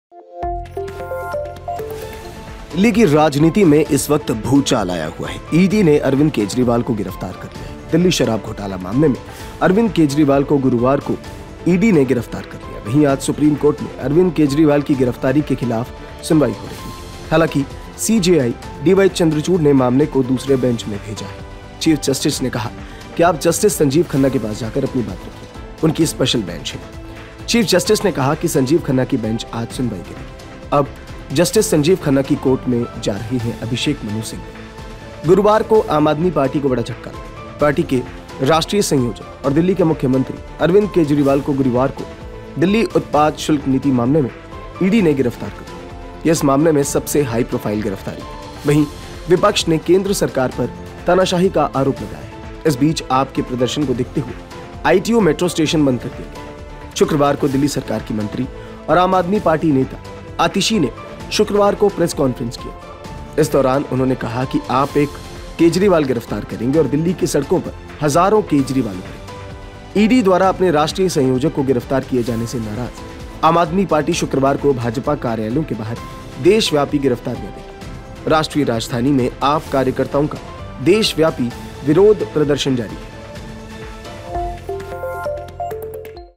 ED Arrested Arvind Kejriwal: केजरीवाल गिरफ्तार, सुप्रीम कोर्ट में गुहार
Delhi Sharab नीति घोटाला दिल्ली के सीएम अरविंद केजरीवाल के लिए मुसीबत बन गया है। आम आदमी पार्टी के संयोजक अरविंद केजरीवाल को फिलहाल दिल्ली हाईकोर्ट से कोई राहत नहीं मिली।